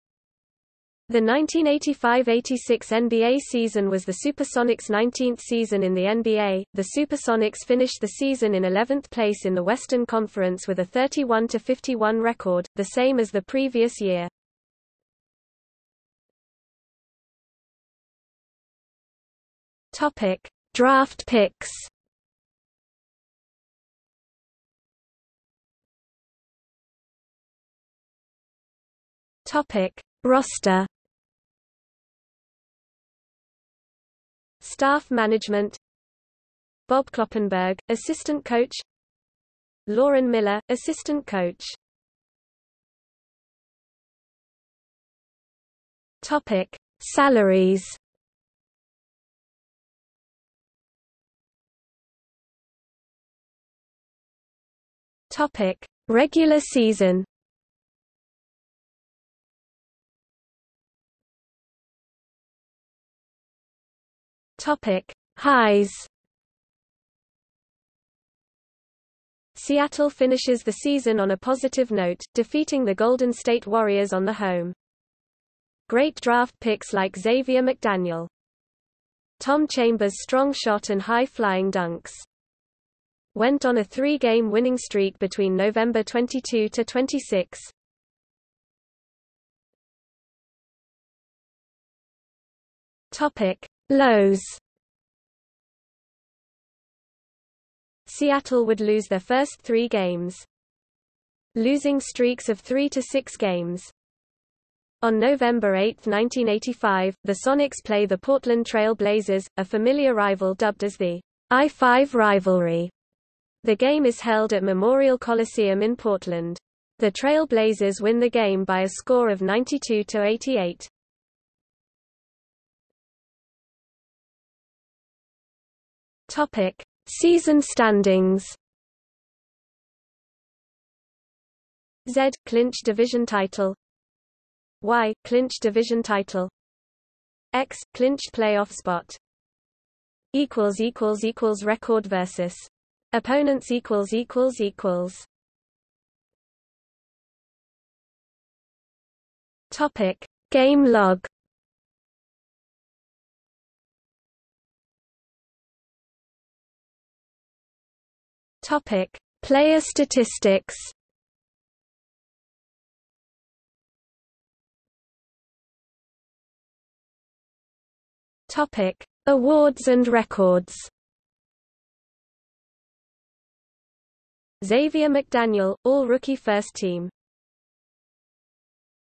1985–86 Seattle SuperSonics season | Wikipedia audio article
00:00:16 1 Draft picks
00:00:25 2 Roster
00:00:42 2.1 Salaries
00:00:51 3 Regular season
00:01:01 3.1 Highs
00:01:29 3.2 Lows
00:02:02 3.3 Season standings
00:02:20 3.4 Record vs. opponents
00:02:26 3.5 Game log
00:02:35 4 Player statistics
00:02:44 5 Awards and records

- Socrates

SUMMARY
The 1985–86 NBA season was the SuperSonics' 19th season in the NBA.The SuperSonics finished the season in eleventh place in the Western Conference with a 31–51 record, the same as the previous year.